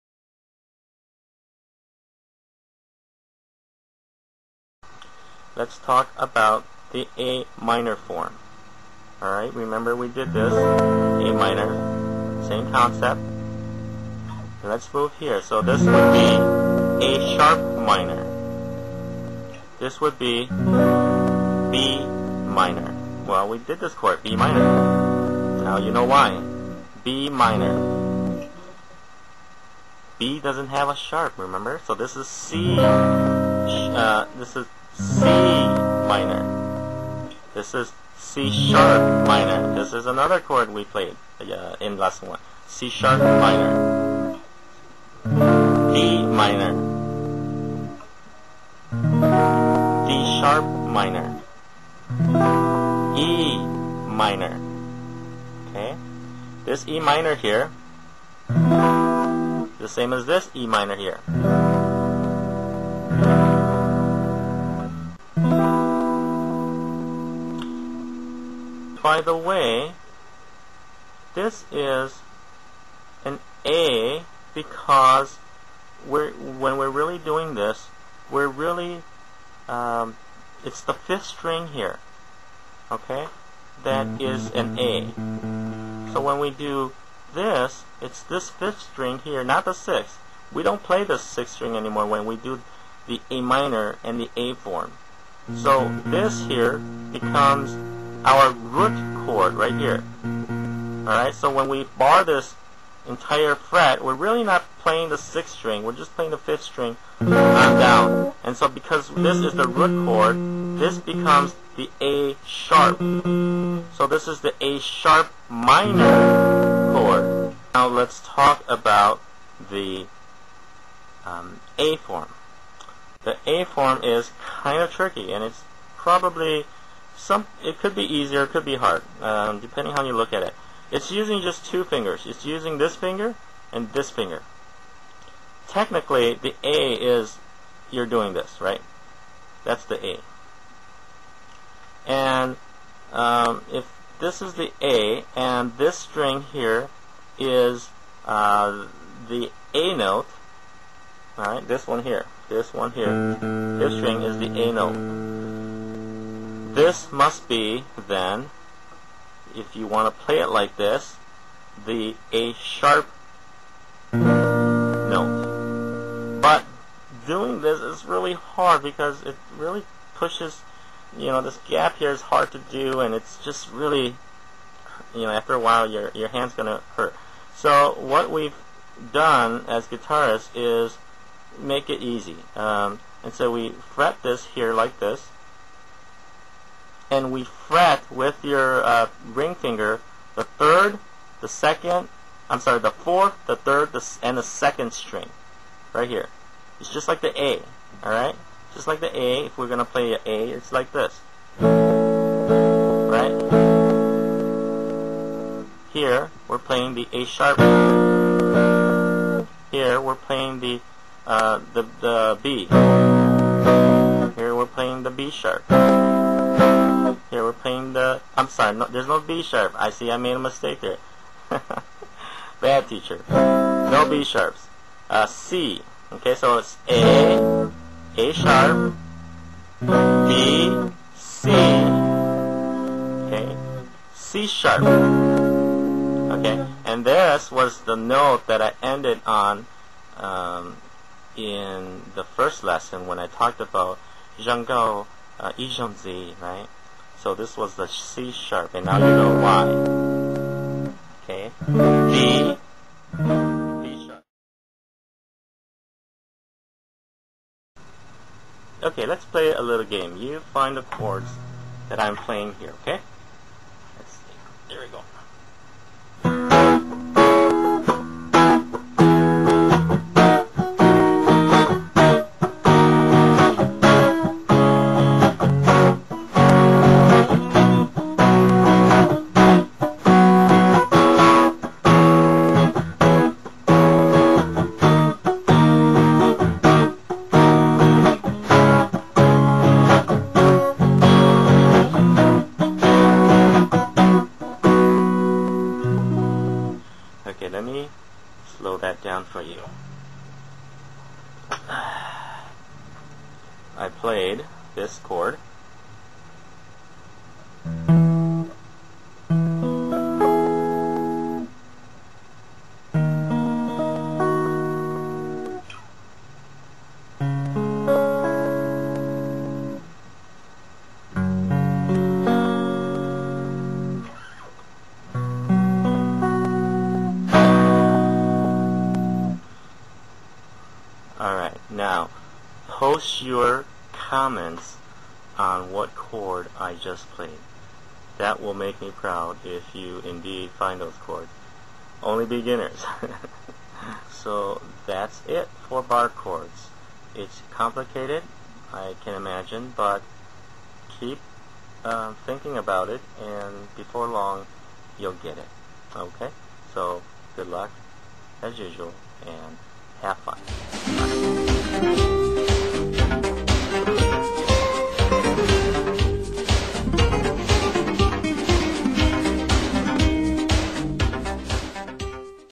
Stain's guitar lesson #2: Barre Chords -- PART II
Part 2 of 2: Learn how to do other guitar chords with the "barre".